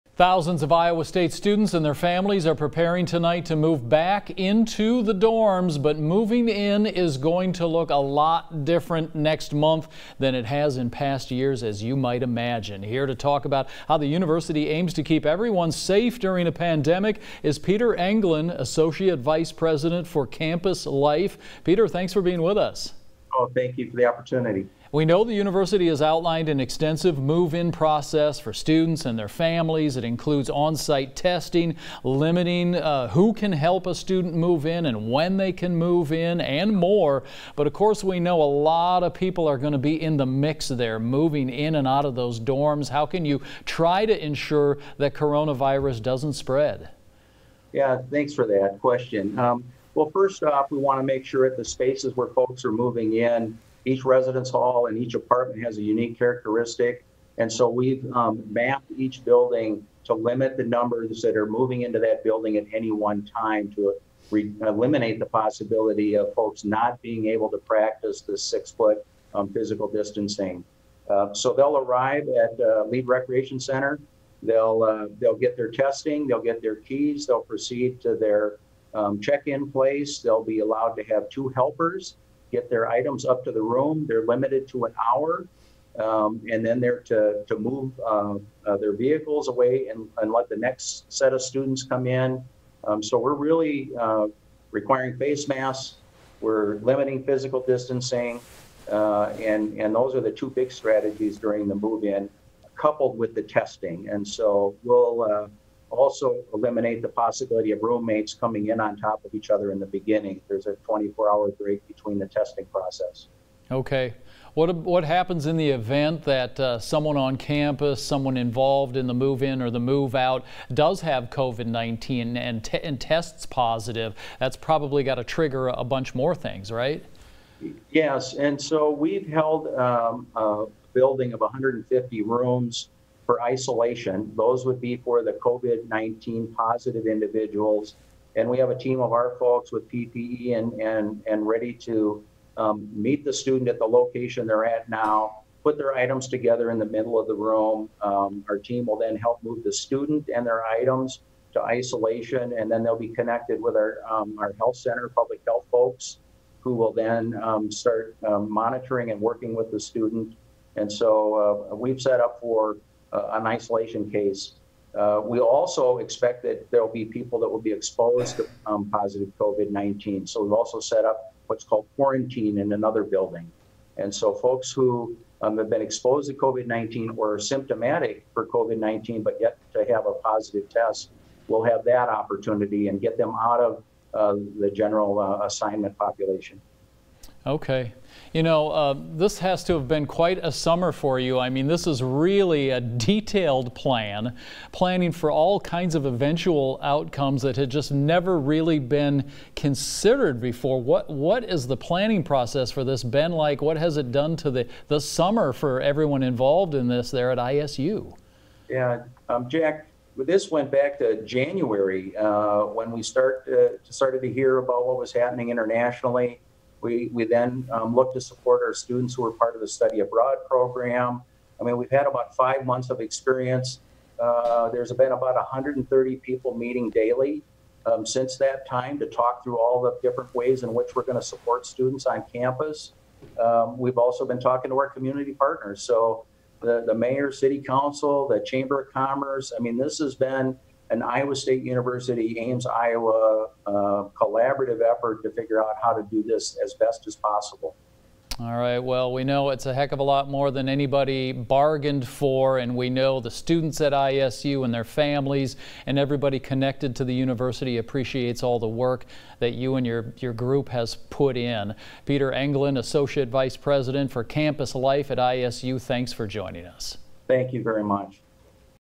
Here's what Iowa State University students need to know before moving into dorms
Thousands of Iowa State students and their families are preparing tonight to move back into the dorms, but moving in is going to look a lot different next month than it has in past years.

Here to talk about how the university aims to keep everyone safe during a pandemic is Peter Englin, Associate Vice President for Campus Life.